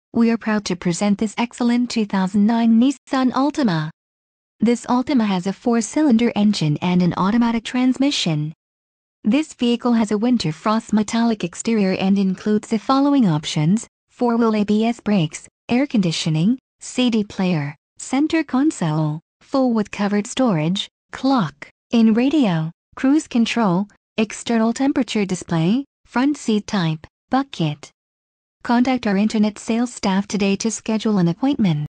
2009 Nissan Altima Pensacola FL Frontier Motors
Frontier Motors
230 Beverly Parkway

2009 Nissan Altima 2.5 S
Engine: L4, 2.5L
Trans: Automatic
Color: Winter Frost Metallic
Mileage: 20,140